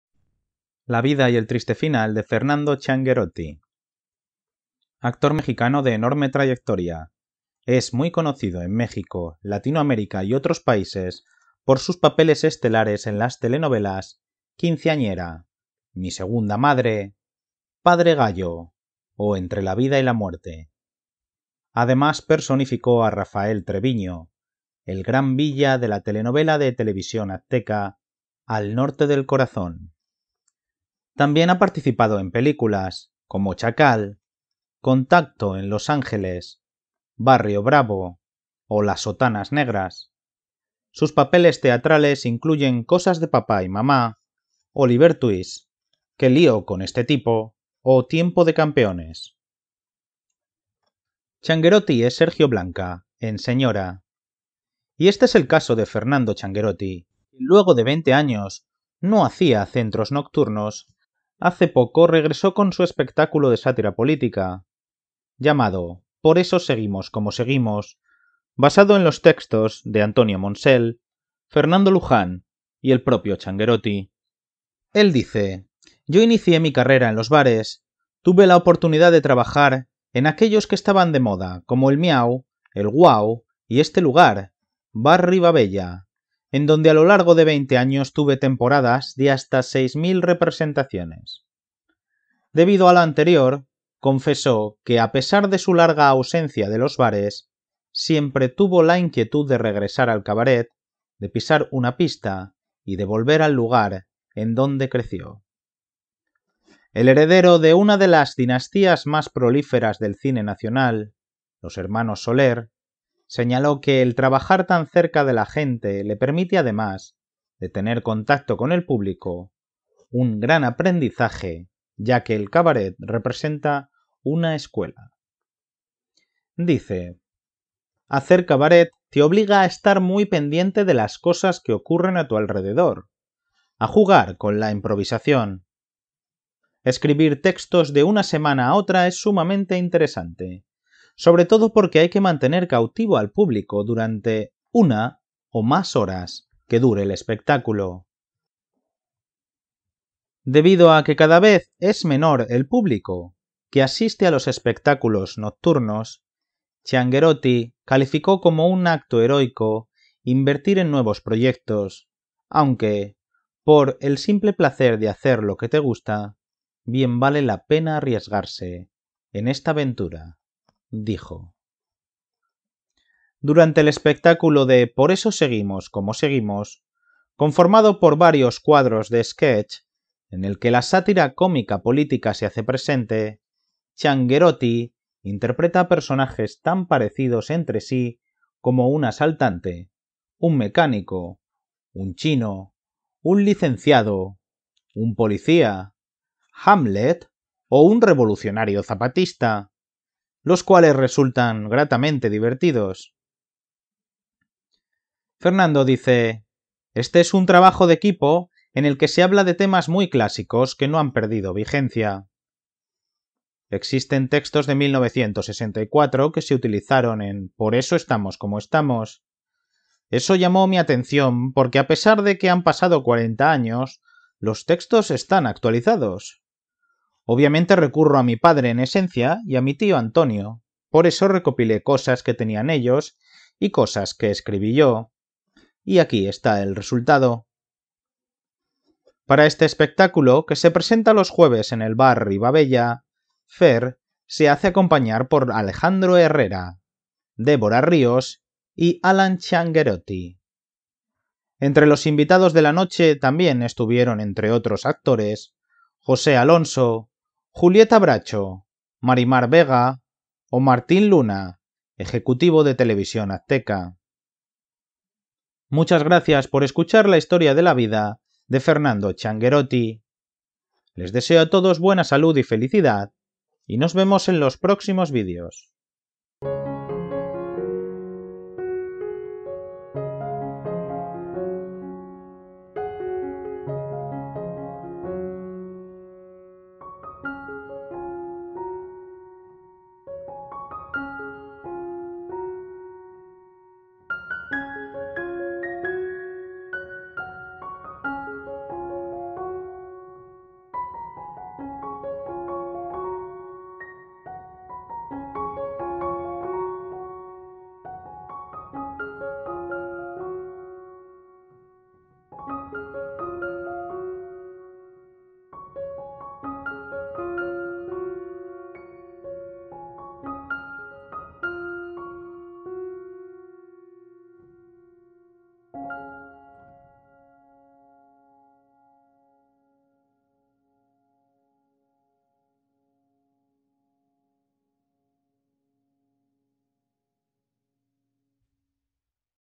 La Vida y El Triste Final de Fernando Ciangherotti
Mini biografía de la vida de Fernando Ciangherotti. Fernando Ciangherotti (Ciudad de México; 6 de septiembre de 1959) es un actor mexicano de cine, televisión y teatro, destacado notablemente por sus roles de antagonista en telenovelas de Televisa y de TV Azteca. Proveniente directo de la Dinastía Soler. Es hijo del actor Fernando Luján y hermano mayor de las actrices Vanessa Ciangherotti, Cassandra Ciangherotti y el director Fernando Canek. Además de Laura Ciangherotti, Alejandra Ciangherotti, Valeria Ciangherotti, Carlos ciangherotti, Sasha Ciangherotti, Bernardo Larraguivel y Franco Paolo.